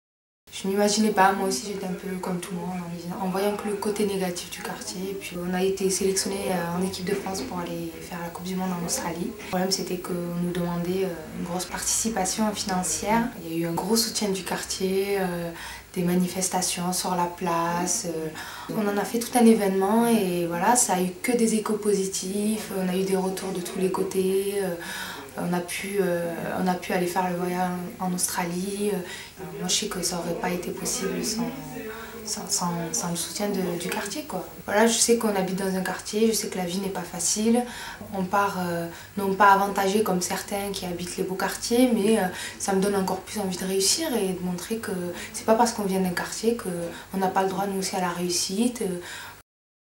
nothingtoulouse_houita_23.mov
short clips of people living in toulouse empalot, the "cité" of toulouse.
say something about your neighborhood! answers are welcome!